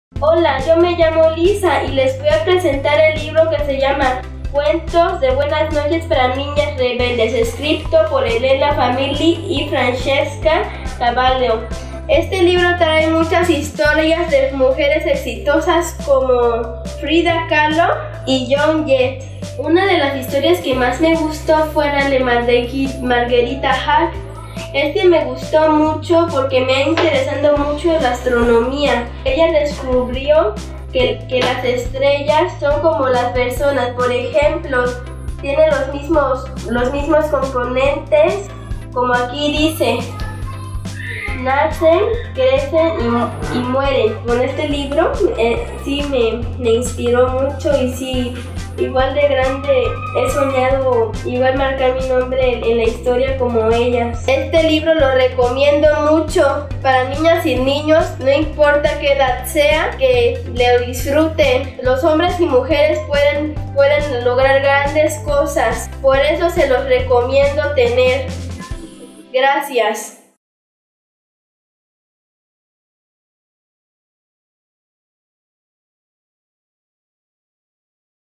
Dinos: ¡Lisa y el libro feminista!
Lisa de Tuxtla Gutiérrez, Chiapas,  nos habla sobre un libro que la inspira a dejar una huella en la historia.

*Episodio 150